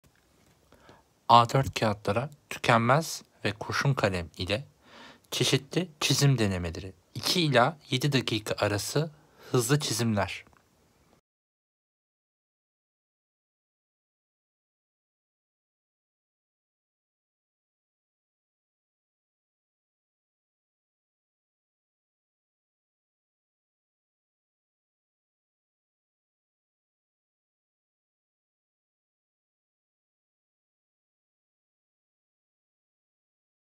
Figür Sketch Çalışmaları - A4 Kağıt
A4 Kağıtlara Tükenmez Kalem ve 3B desen kalemi ile hızlı çizim